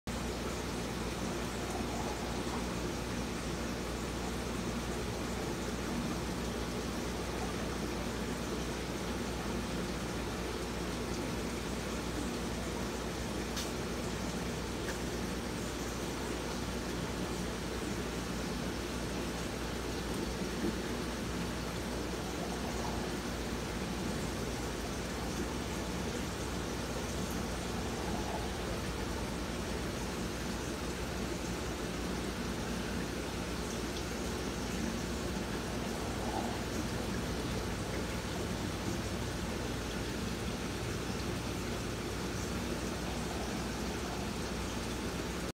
Pair of Convicts & Fry
Courtesy of my LFS. Protecting their babies and keeping all other fish on the other side of the aquarium.